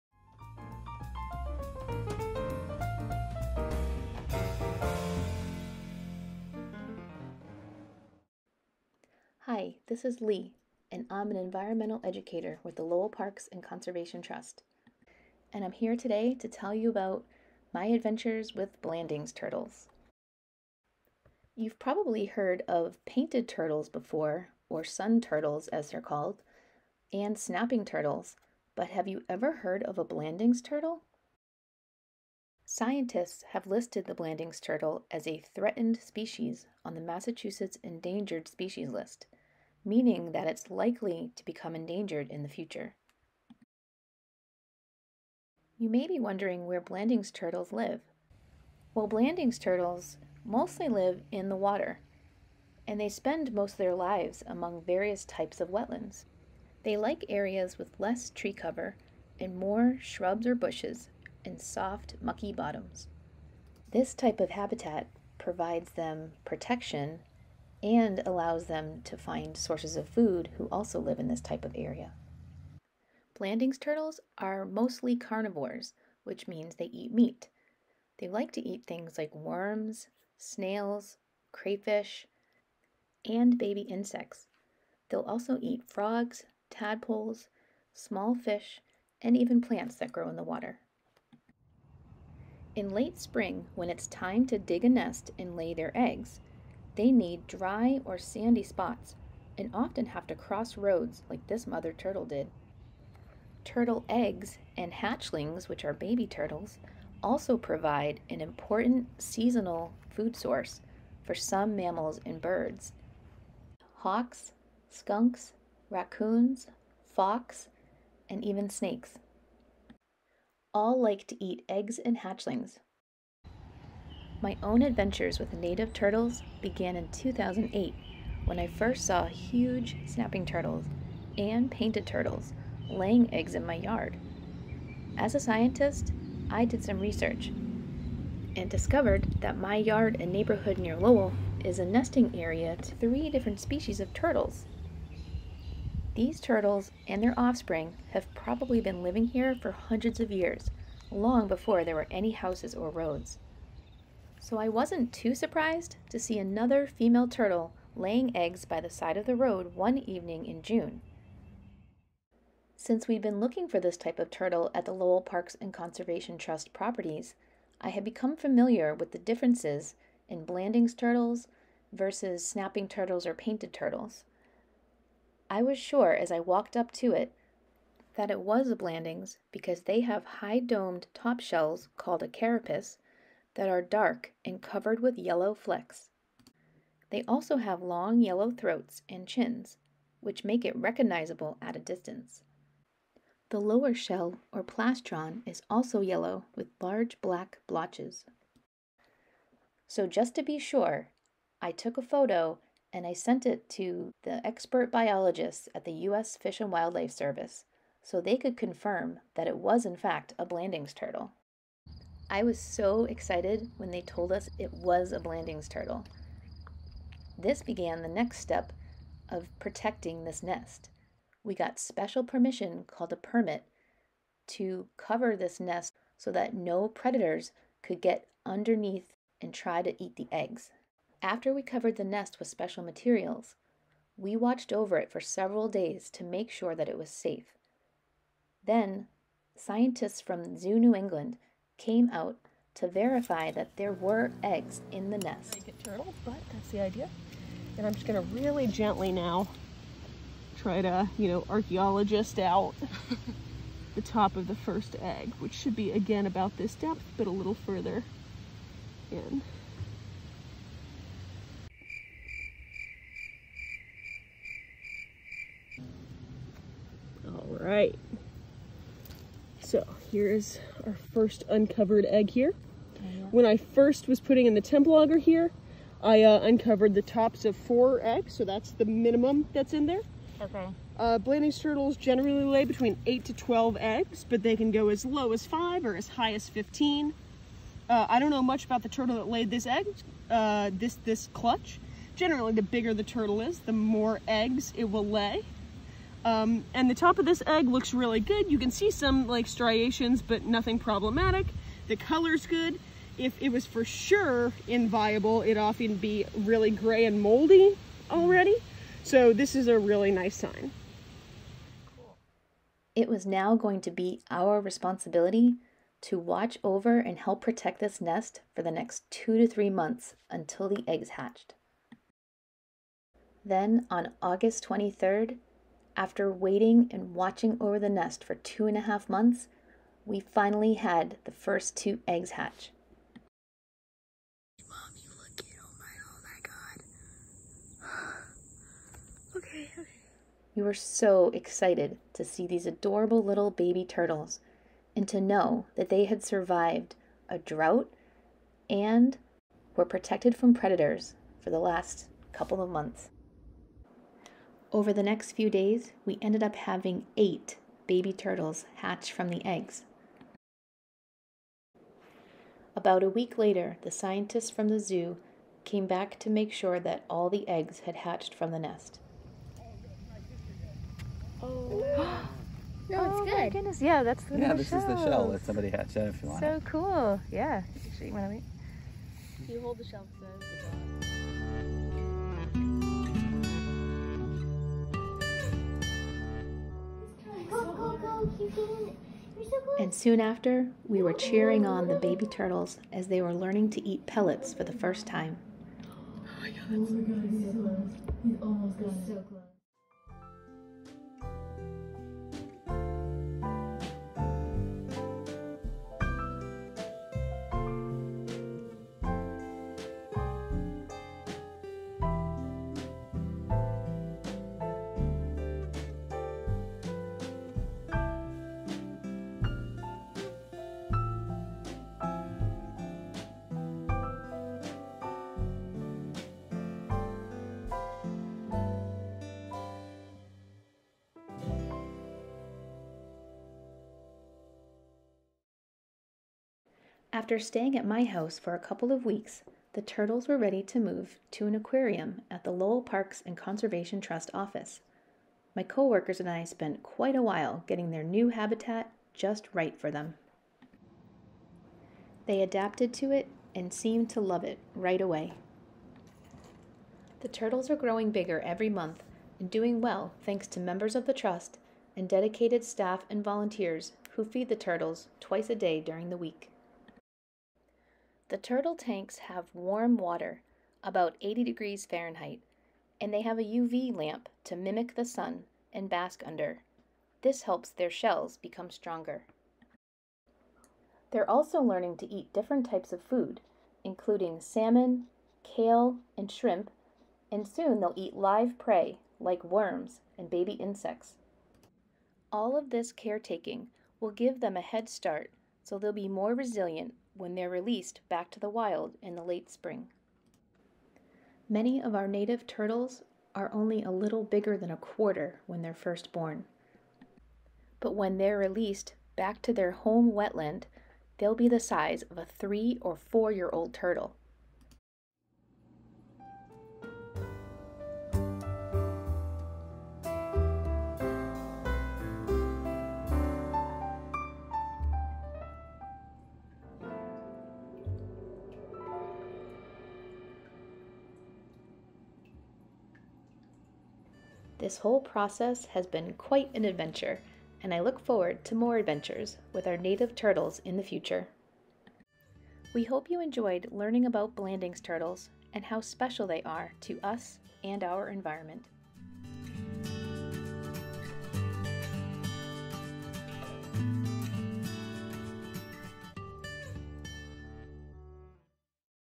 Adventures with Blanding's Turtles
This is the story of how LP&CT came to raise 8 threatened Blanding's turtle hatchlings.